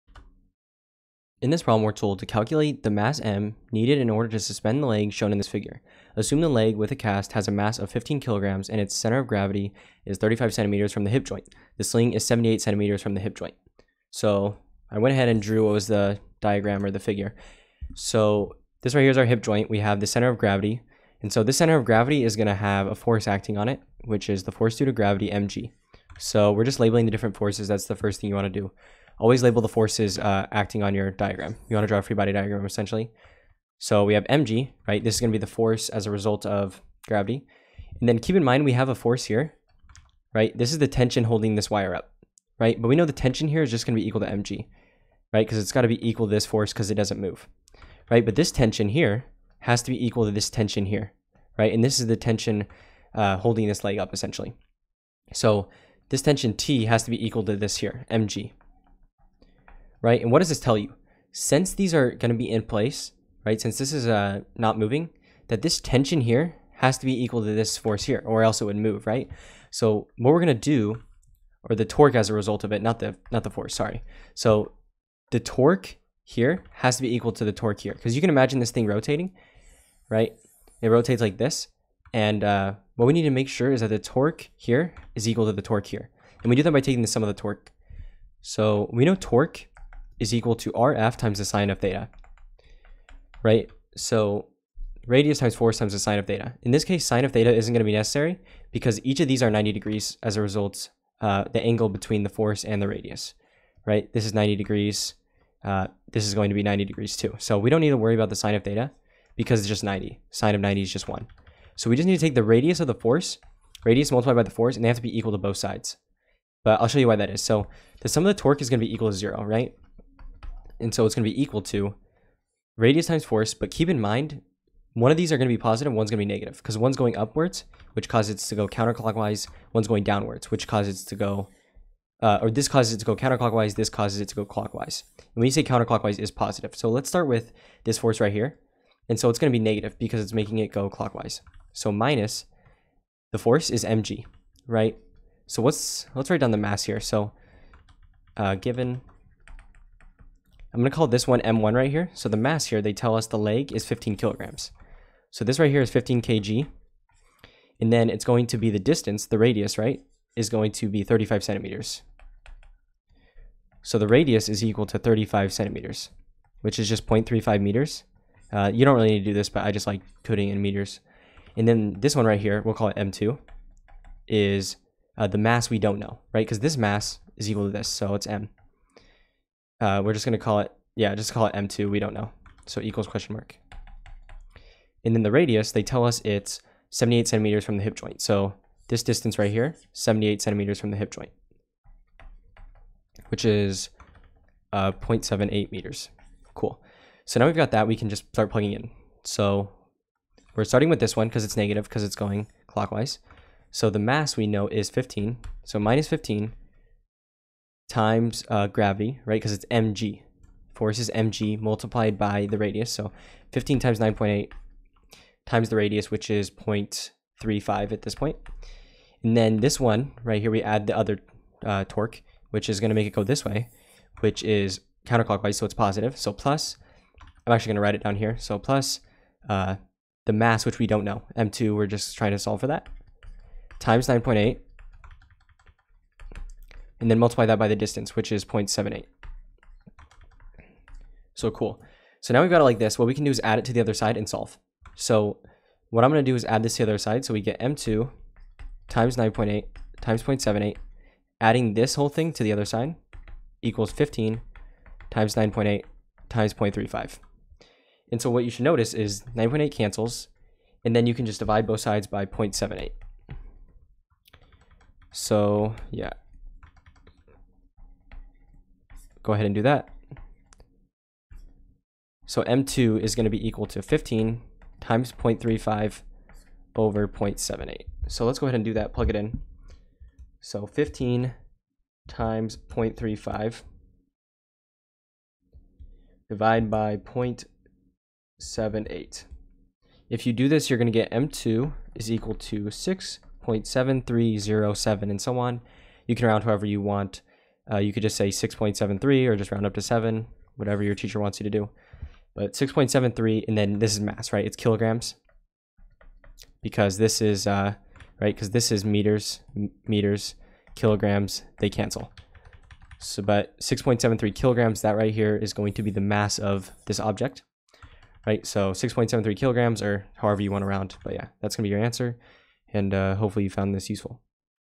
Calculate the mass in needed in order to suspend the leg shown in Fig. 12-47. Assume the leg (cast)
Calculate the mass m needed in order to suspend the leg shown in Fig. 12-47. Assume the leg (with cast) has a mass of 15.0 kg, and its CG is 35.0cm from the hip joint: the sling is 78.0 cm from the hip joint. .